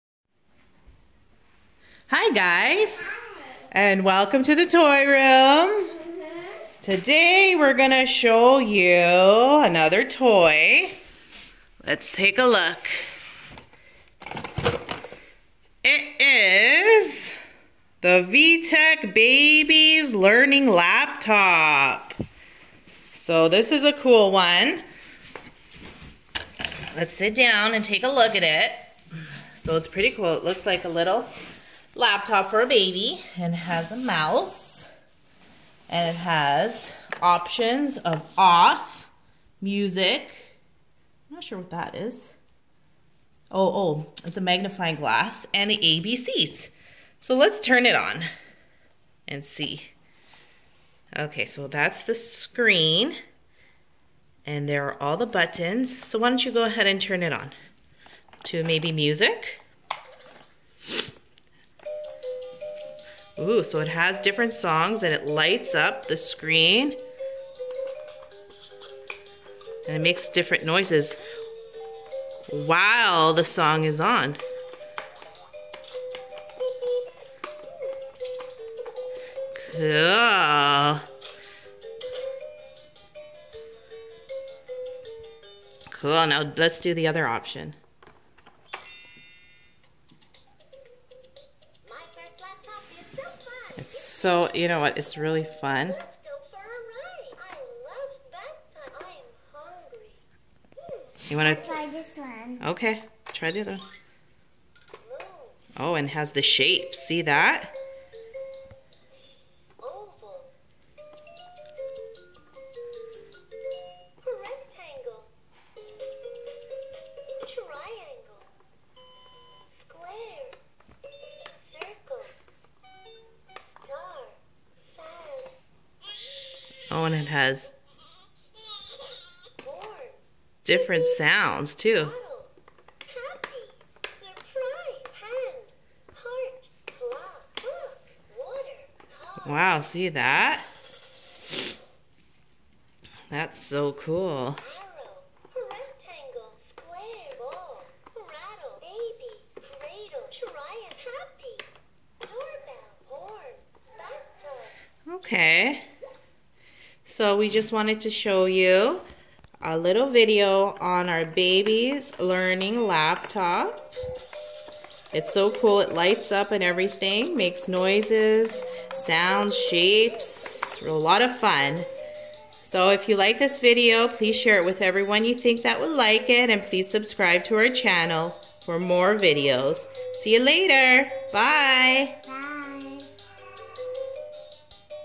VTech Baby's Learning Laptop Fun Demonstration and Review
VTech Baby's Learning Laptop Fun Demonstration and Review: In this video we look at our baby's learning laptop from vtech. It is so much fun to play with!  It lights up and makes a lot of sounds and music.  We love it!